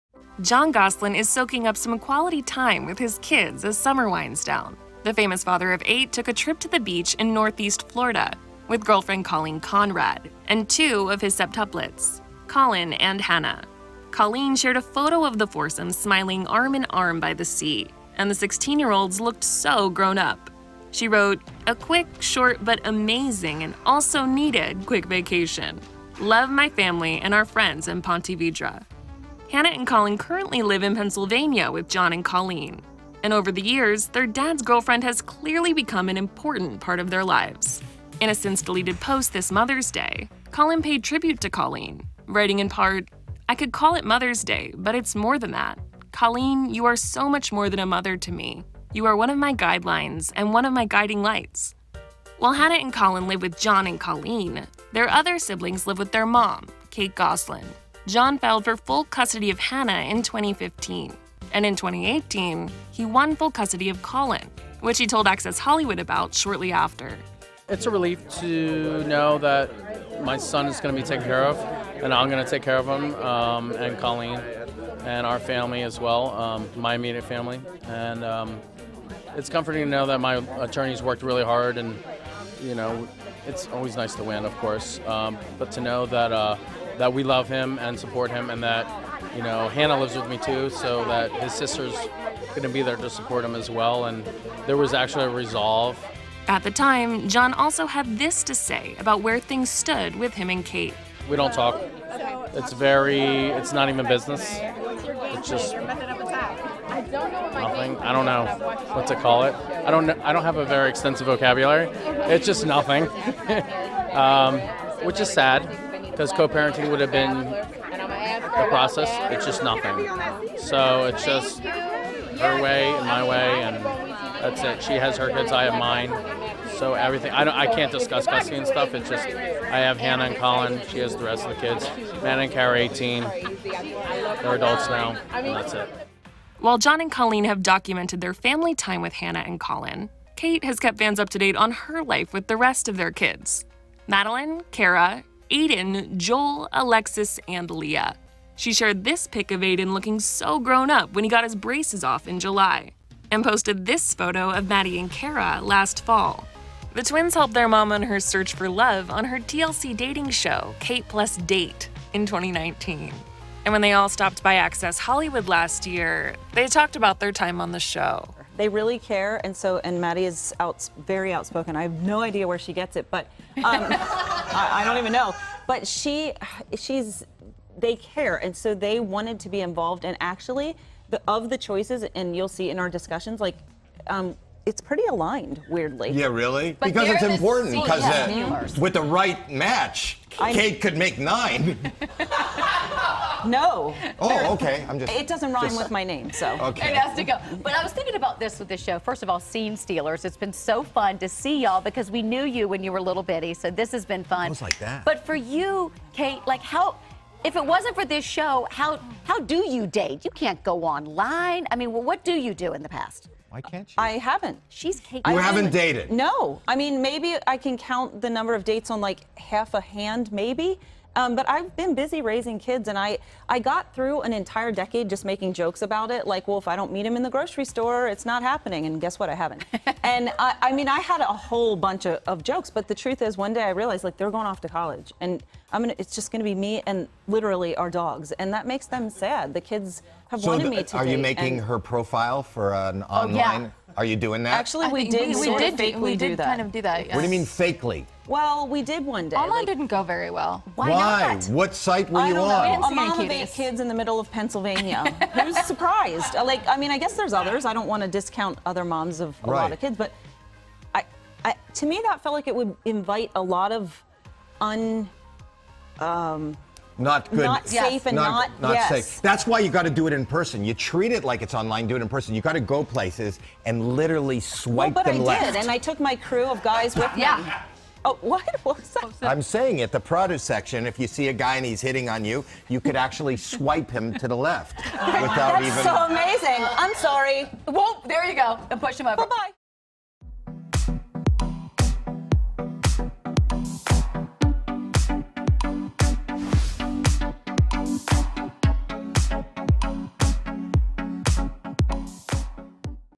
Collin & Hannah Gosselin Look So Grown Up In Family Vacation Photo
Jon Gosselin is soaking up some quality time with his kids as summer winds down! The famous father of eight took a trip to the beach in Florida, with girlfriend Colleen Conrad and two of his septuplets, Collin and Hannah. Colleen shared a photo of the foursome smiling arm in arm by the sea, and wrote, "A quick, short but amazing and also needed quick vacation! Love my family and our friends in Ponte Vedra!!!"

Collin & Hannah Gosselin Look So Grown Up In New Photo